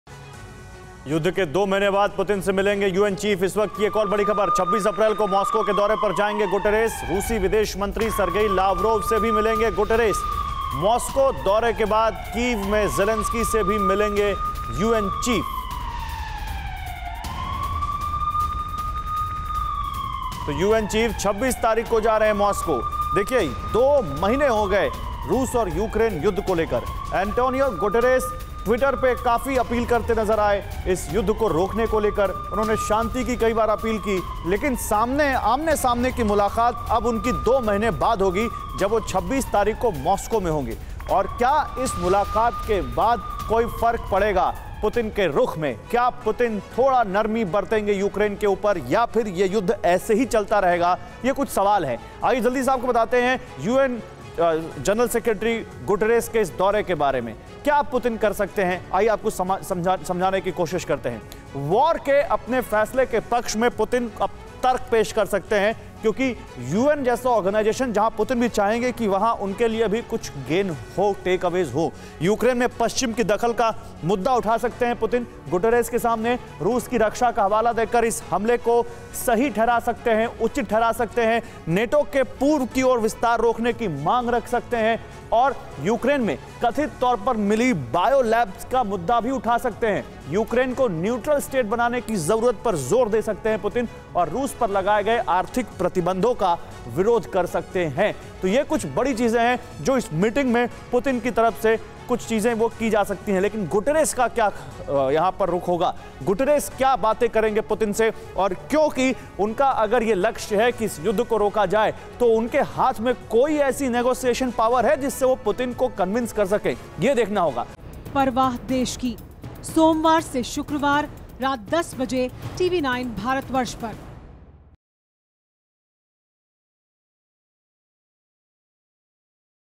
Russia Ukraine Crisis: 26 अप्रैल को Moscow के दौरे पर जाएंगे Antonio Guterres | WAR
रूस और यूक्रेन के बीच जंग लगातार जारी है, रूस लगातार हमले कर रहा है, इसी बीच युद्ध के 2 महीने बाद पुतिन से मिलेंगे UN चीफ. 26 अप्रैल को मॉस्को के दौरे पर जाएंगे गुटेरेस. रुसी विदेश मंत्री सर्गेई लावरोव से भी मिलेंगे गुटेरेस.
The war between Russia and Ukraine is continuing, Russia is continuously attacking, meanwhile the UN Chief will meet Putin after 2 months of the war. Guterres will visit Moscow on April 26. Guterres will also meet with Russian Foreign Minister Sergei Lavrov.

Credit